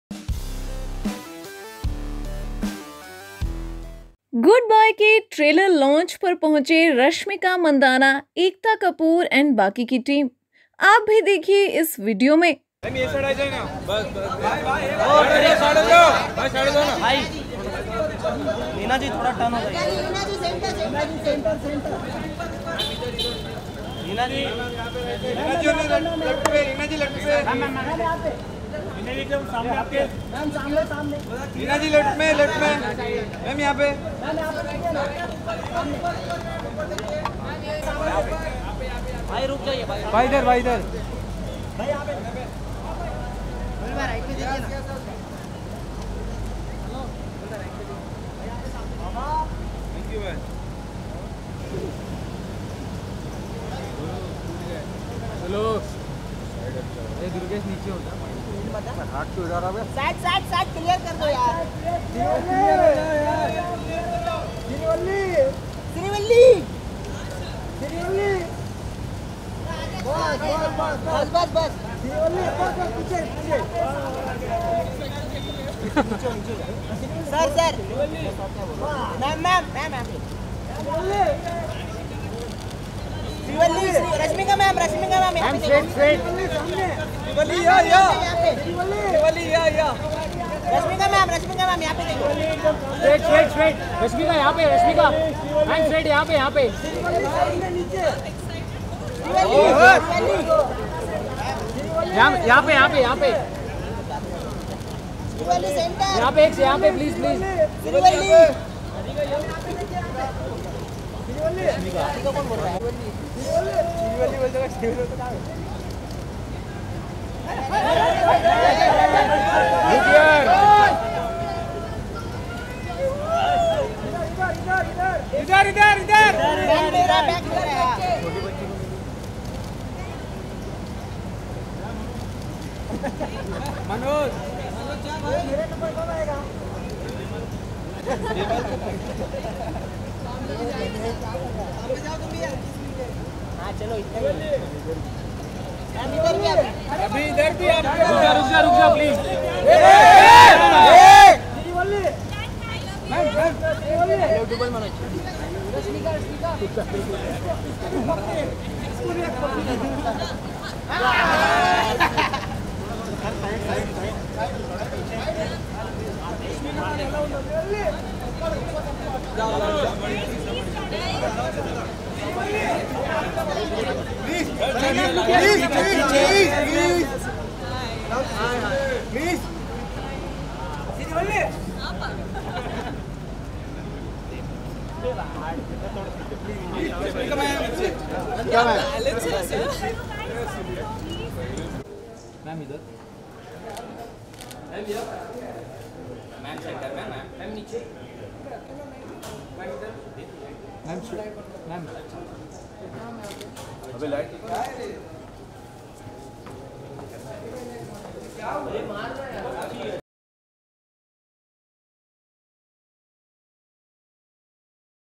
Rashmika Mandanna Arrived At Trailer Launch Of Film Good Bye
RashmikaMandanna Arrived At Trailer Launch Of Film Good Bye